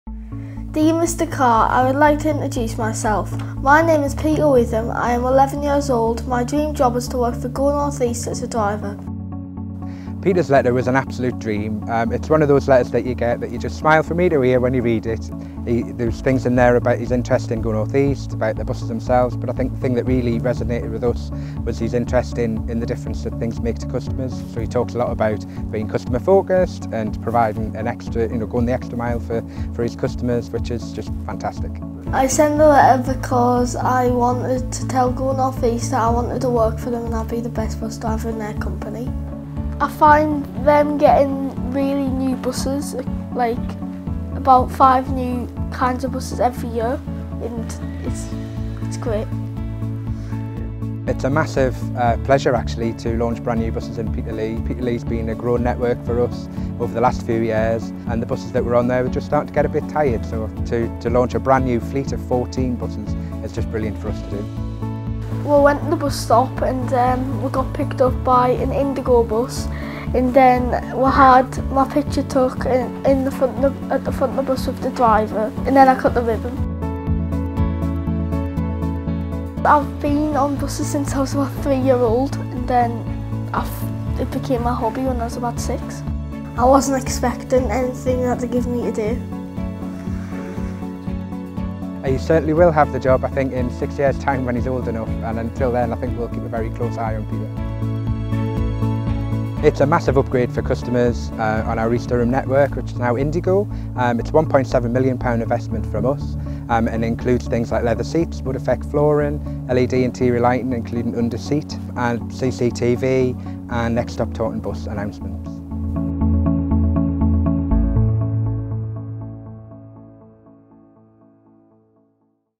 Go North East schoolboy super fan launches £1.7m indiGo
When 11-year-old schoolboy Peter Witham wrote to our managing director to ask for a bus driver job, he didn't even think he would get a reply.

But his letter was so fantastic, we did better than that and made him a bus VIP for the day.